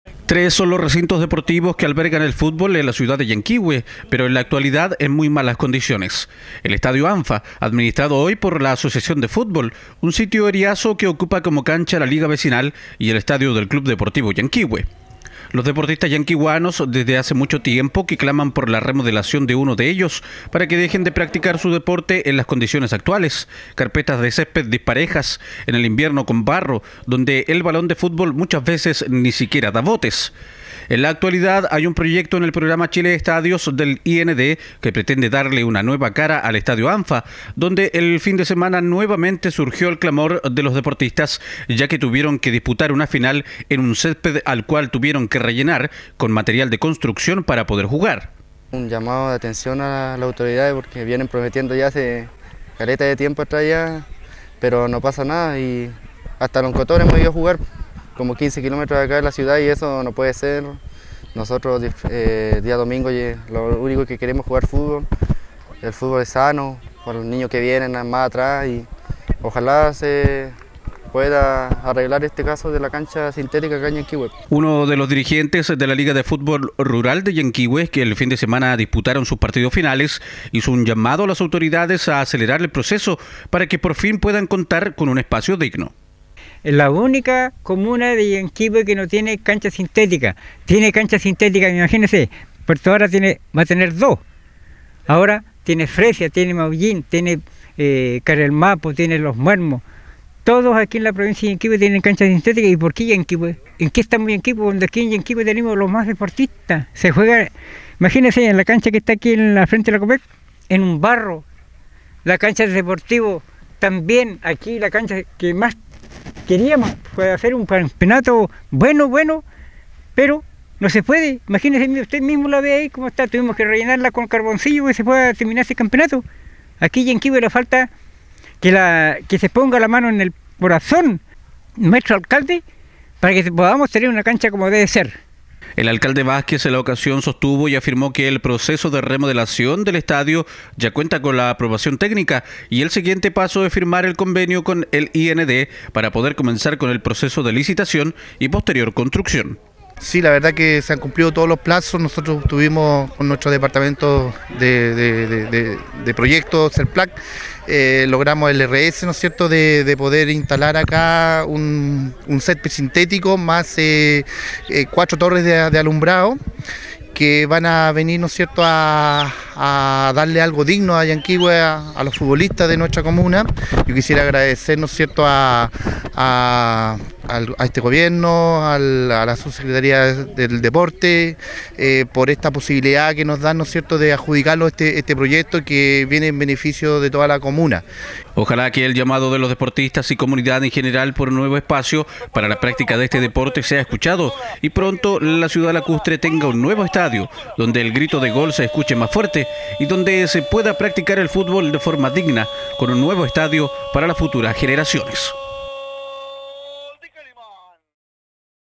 EL TAN ANHELADO ESTADIO PARA LLANQUIHUE
Alcalde Vasquez asegura que el próximo paso es la firma del convenio con el IND para comenzar con la licitación y construcción del estadio con cancha sintética para LLanquihue.